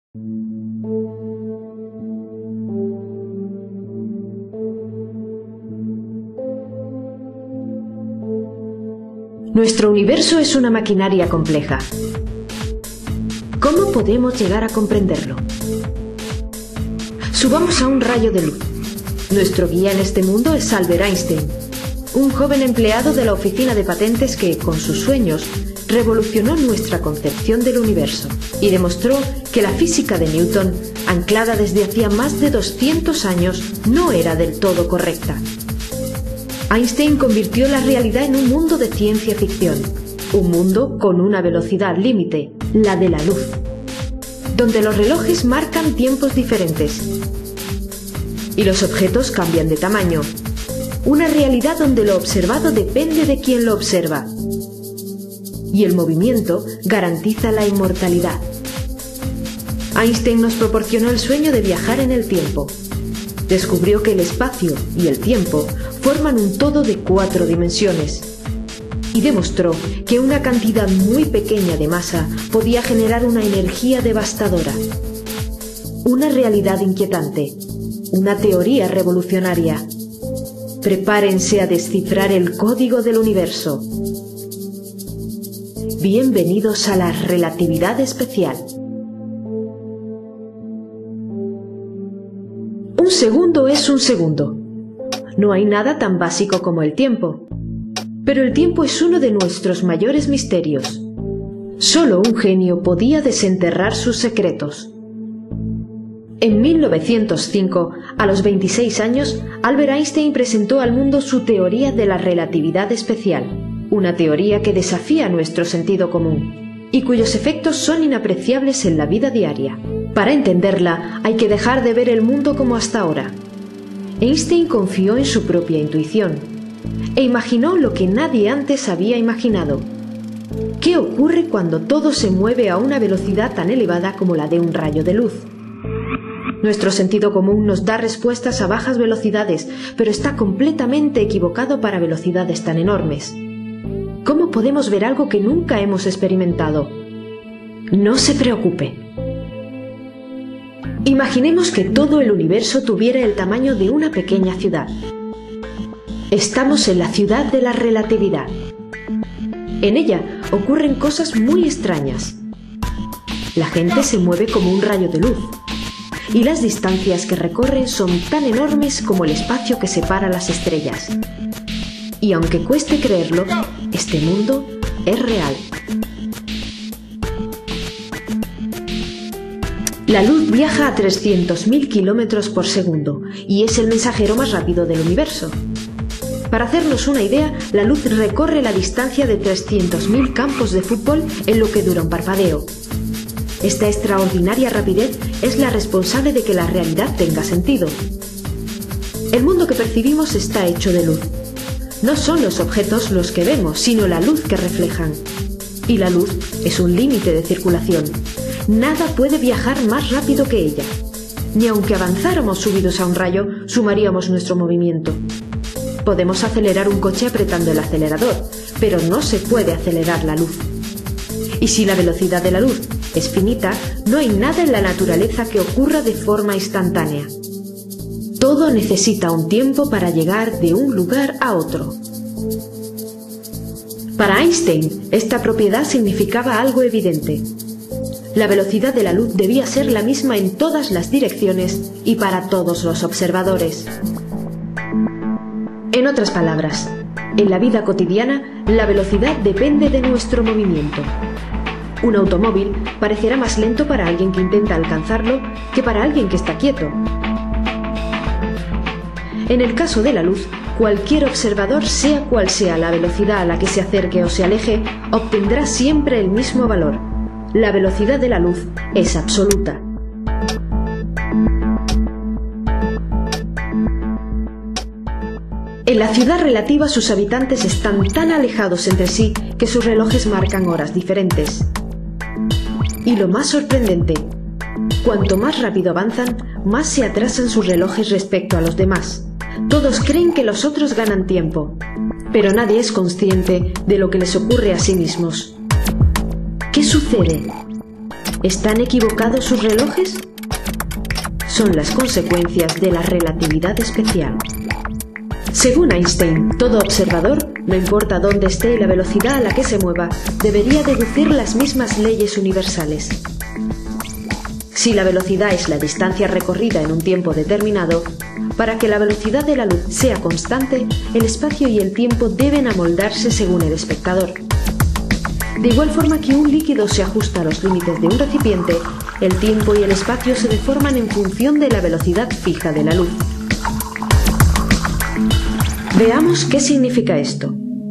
Relatividad Especial (parte 1)
Vídeo divulgativo sobre la Teoría de la Relatividad de Einstein realizado por el Instituto de Astrofísica de Canarias para conmemorar el centenario de la teoría en la celebración del Año Mundial de la Física 2005.